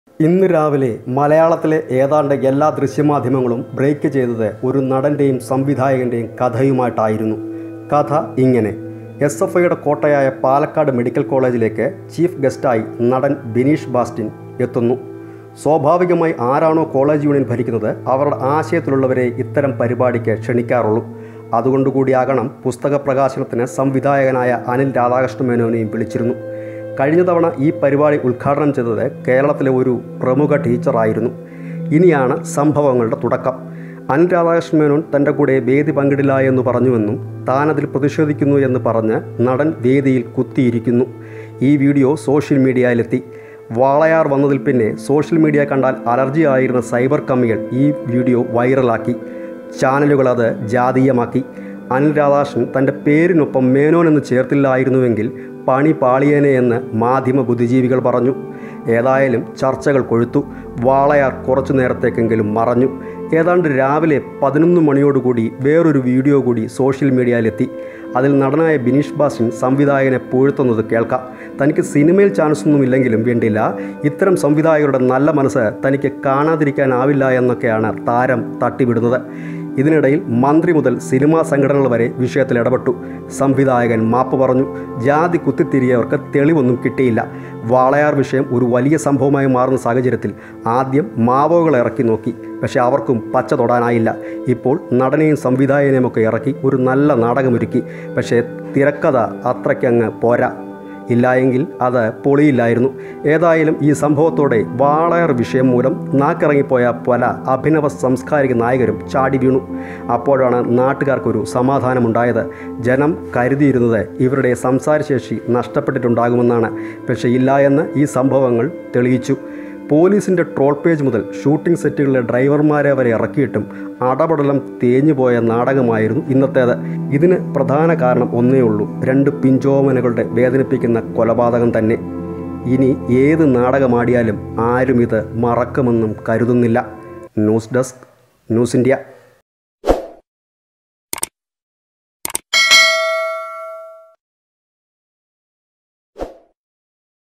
സിനിമാക്കരുടെ ബ്രേക്കിഗ് ചീറ്റിയ കഥ | Bineesh Bastin Anil Radhakrishnan Menon Dispute
വാളയാറു ഒതുക്കാന്‍ ഒരുക്കിയ തിരകഥ ഇങ്ങനെ; സിനിമാക്കരുടെ ബ്രേക്കിഗ് ചീറ്റിയ കഥ